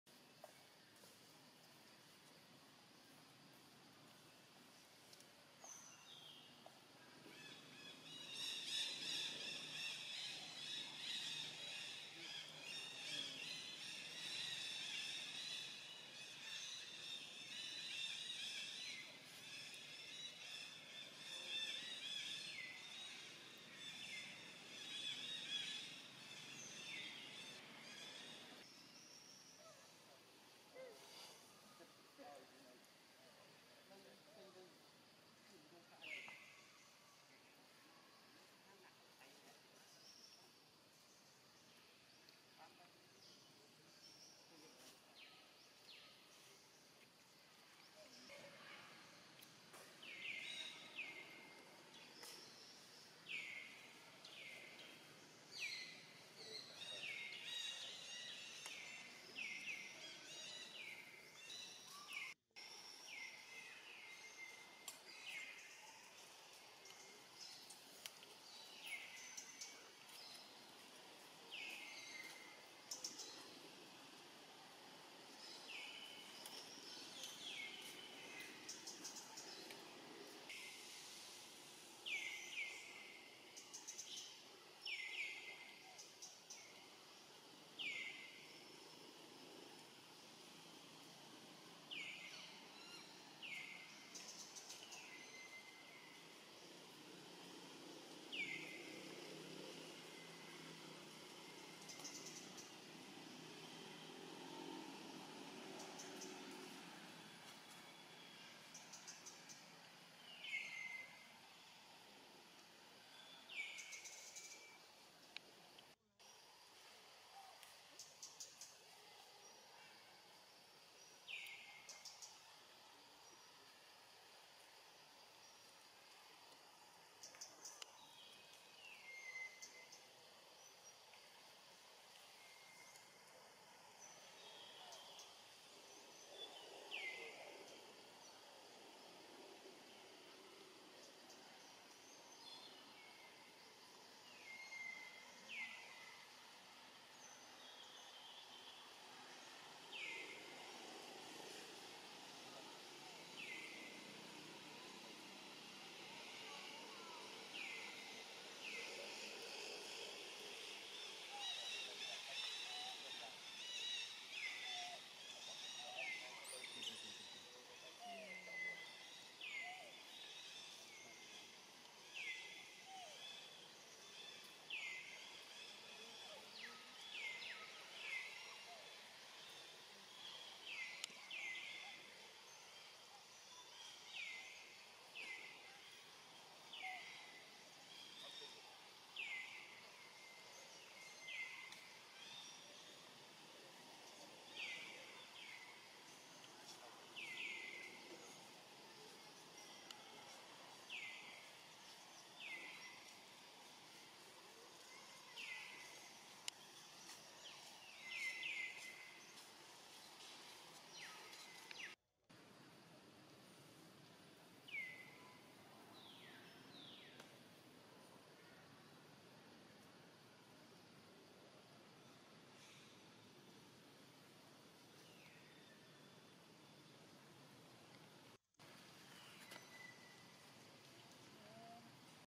Happiness Moments Ever Of Libby's Family, LEO and Rainbow Having Fun Together, Libby Site and Wac
Happiness Moments Ever Of Libby's Family, LEO and Rainbow Having Fun Together, Libby Site and Watch, lovely Monkey.mp4